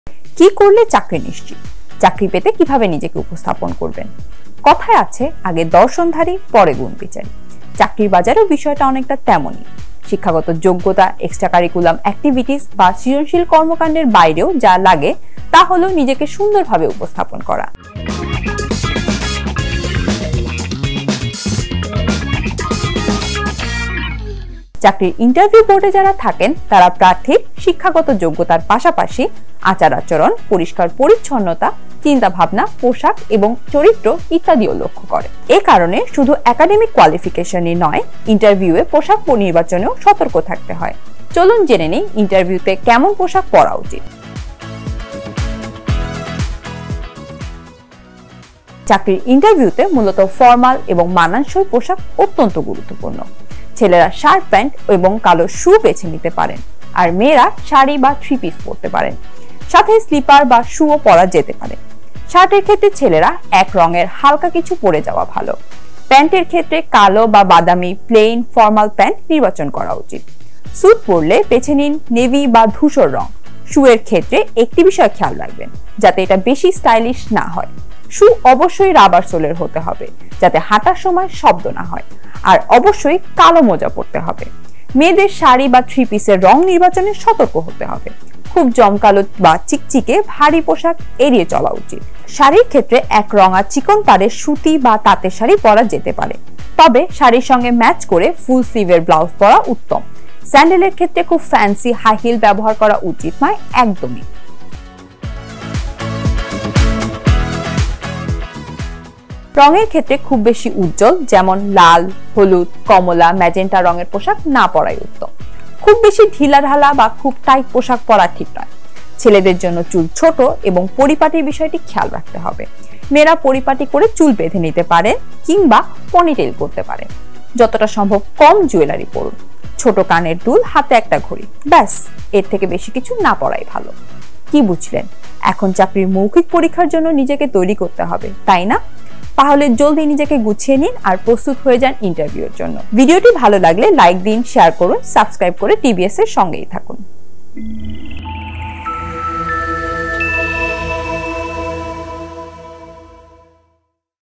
চাকরি পেতে কিভাবে নিজেকে উপস্থাপন করবেন
কথায় আছে, আগে দর্শনধারী, পরে গুণ বিচারী।  চাকরির বাজারেও বিষয়টা অনেকটা তেমনই। শিক্ষাগত যোগ্যতা, এক্সটা কারিকুলাম এক্টিভিটিজ বা সৃজনশীল কর্মকাণ্ডের বাইরেও যা লাগে তা হল নিজেকে সুন্দরভাবে উপস্থাপন করা।